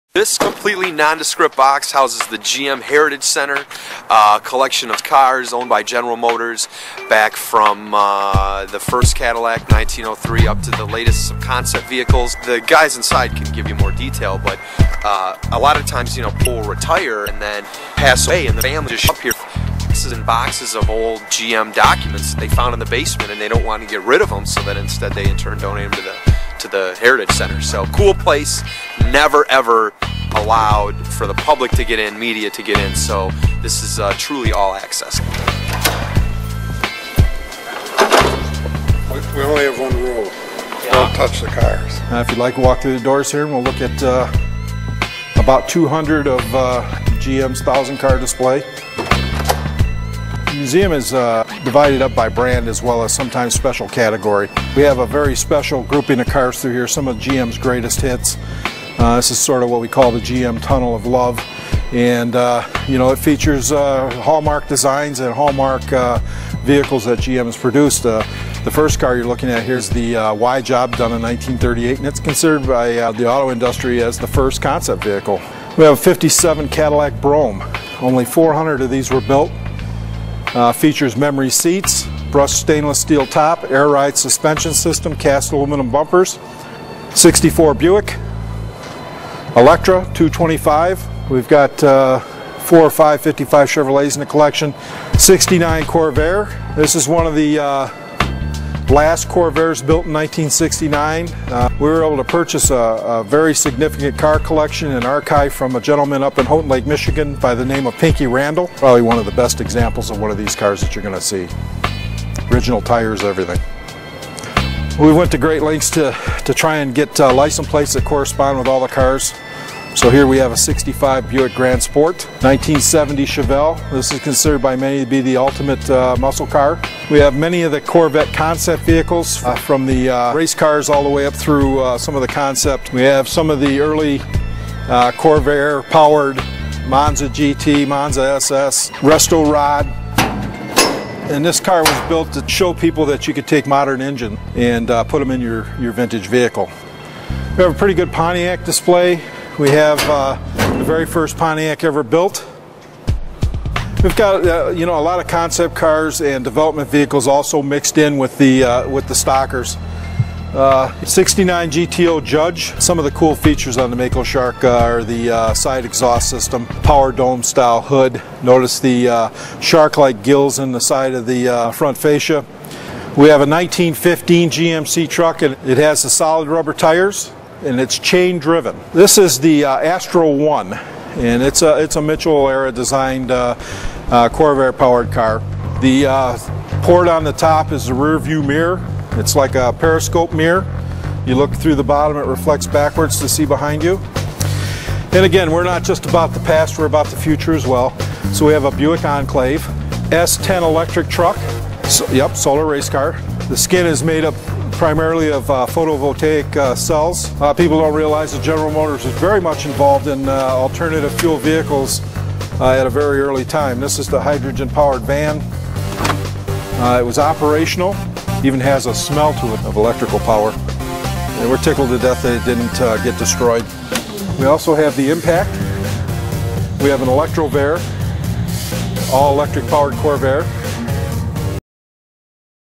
The GM Heritage Center
A rare look inside the GM Heritage Center, which has archived over 1000 vintage, custom, and show cars dating back to the turn of the last century.